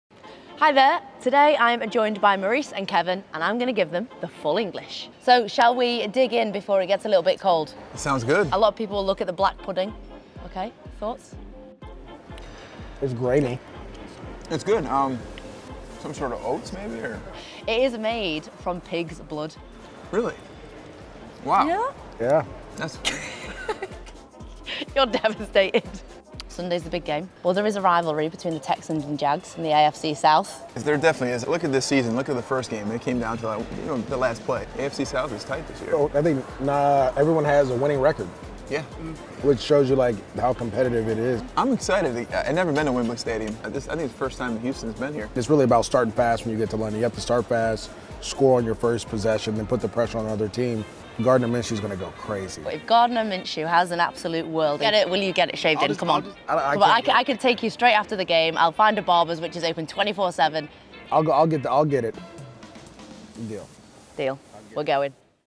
Maurice Jones-Drew & Kevin Walter Try A Full English Breakfast 🍳 NFL London Games | NFL UK
Before the Houston Texans face off against the Jacksonville Jaguars, Maurice Jones-Drew and Kevin Walter try a full English breakfast for the first time.

Follow the NFL academy accounts for the best content from our aspiring UK American football players